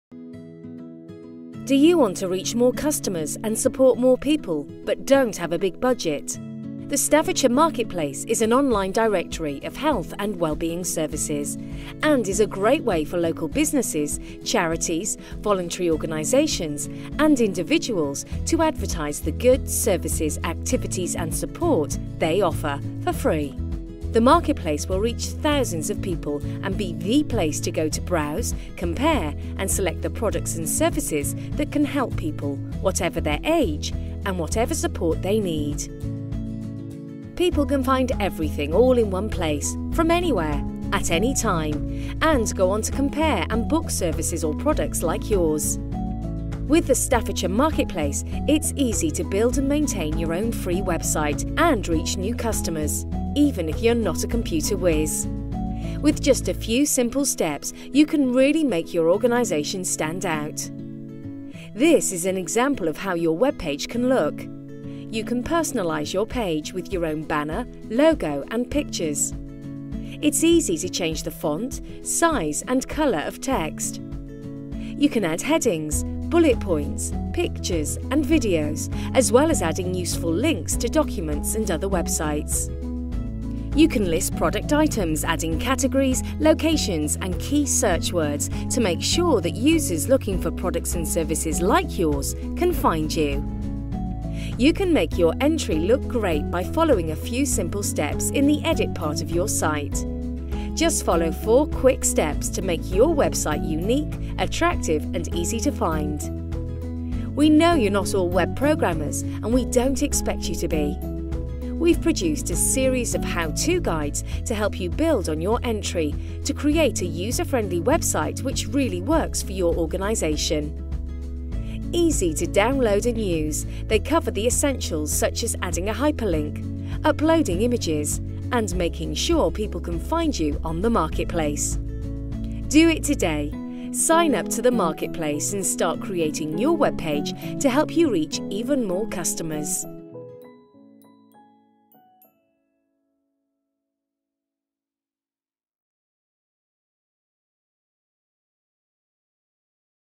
Staffordshire Marketplace HD
The Staffordshire Marketplace is a great way for local businesses, charities, social enterprises and individuals to advertise the range of goods, services, activities and support they offer for residents of Staffordshire and Stoke-on-Trent for free.